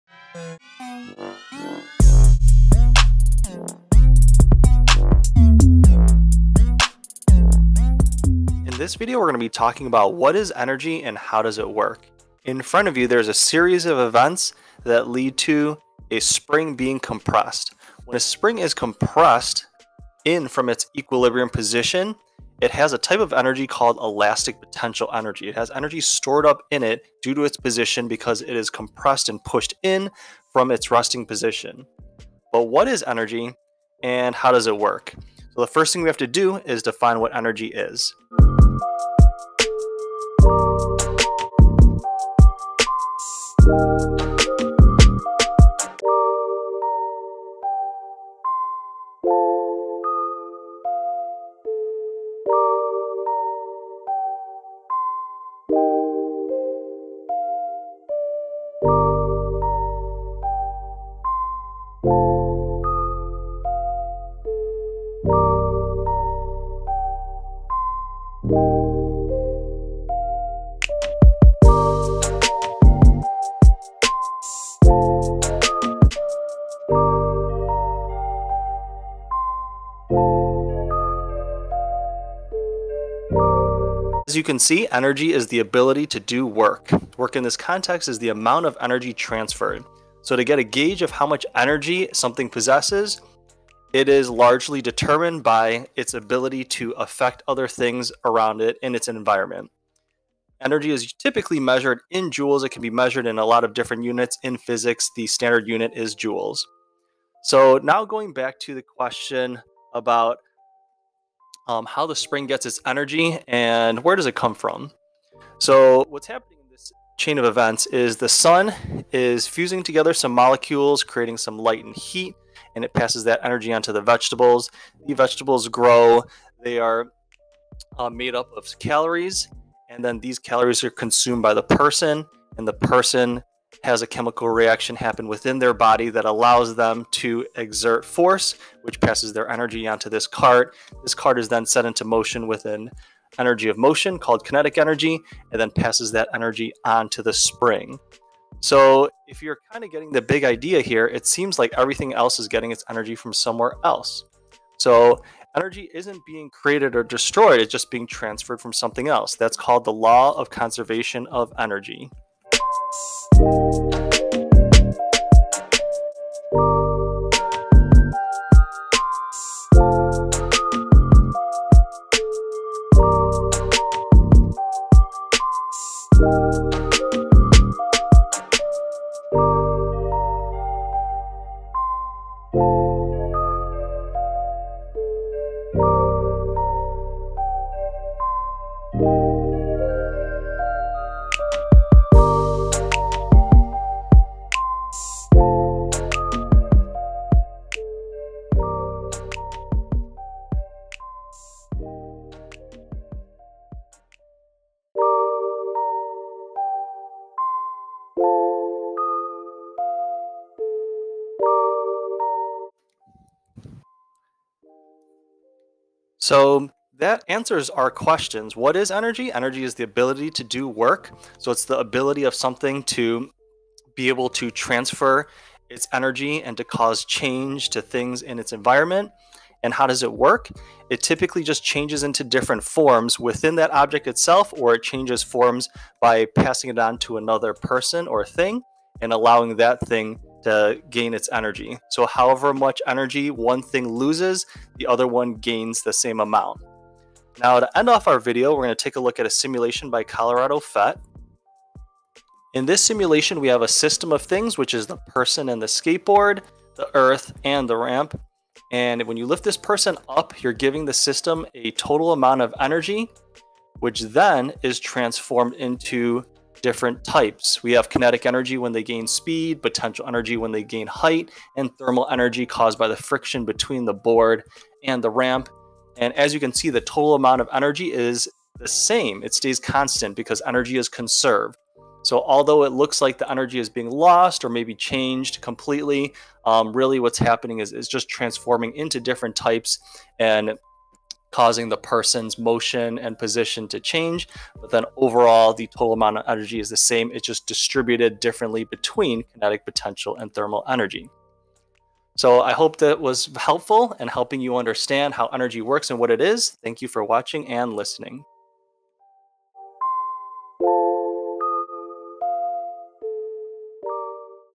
Energy - What is it? How Does it Work?
In this video, an explanation is provided of what energy is and how it is transferred.  Energy, Work, and The Law of Conservation of Energy are defined in this video along with an overview of how objects obtain their energy through the transfer of energy from other things in their environment.